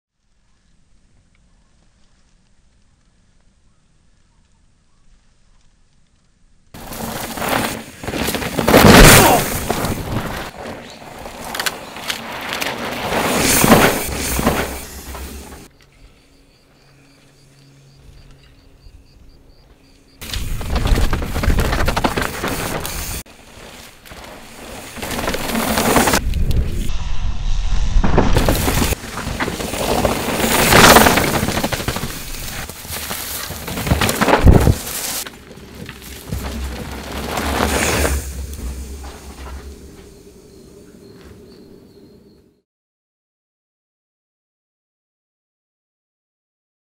Ryan Reish RAW Edit
Ryan and I started filming for a larger project but that has since changed directions so I wanted to upload this raw edit since this footage won't be used for that video. This was a fun shoot though it was my first time filming MTBing at all and we headed into the Santa Barbara Backcountry to grab some clips as Ryan prepares to race the EWS this year.